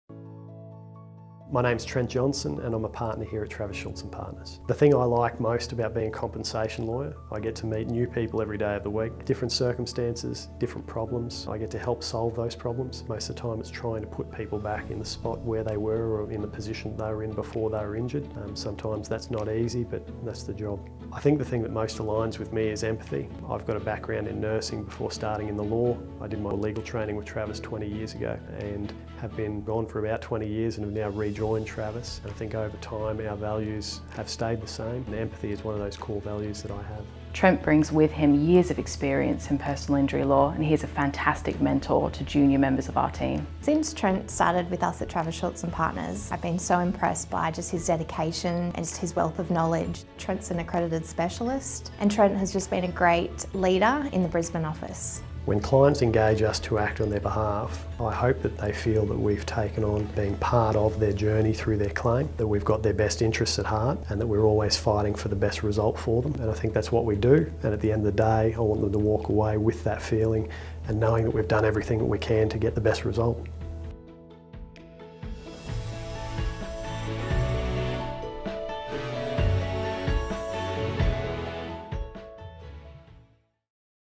Meet Trent | Travis Schultz & Partners | Partner & Brisbane Leader
As well as a Partner and Brisbane Leader, Trent is a Queensland Law Society Accredited Specialist in Personal Injury Law and has acted for thousands of injured claimants. He is an Editor for Lexis Nexis, an online legal publication for Queensland personal injuries, and a long-term member of the Queensland Law Society Accident Compensation Tort Law Committee.

As a former registered nurse, Trent is acutely aware of the impact injury and illness can have upon his clients and their loved ones.

As a vocal advocate for his clients, Trent is motivated to ensure they obtain the justice they deserve.